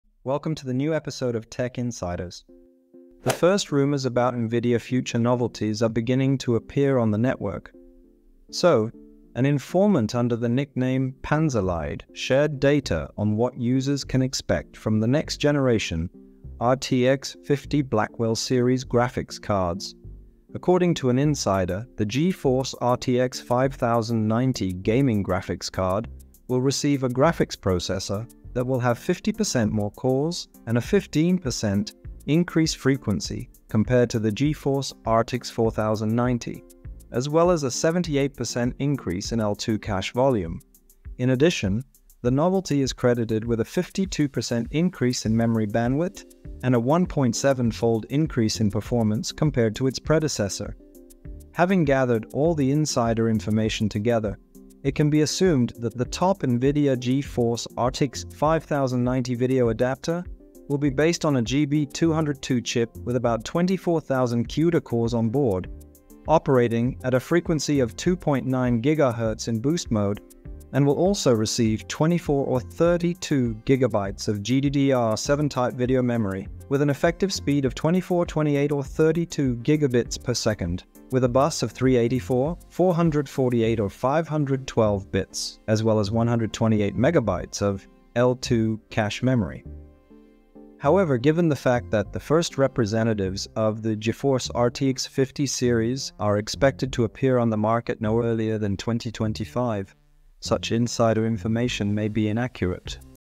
NVIDIA GeForce RTX 5090: First Rumors
Today we will look at the latest tech insiders about NVIDIA GeForce RTX 5090.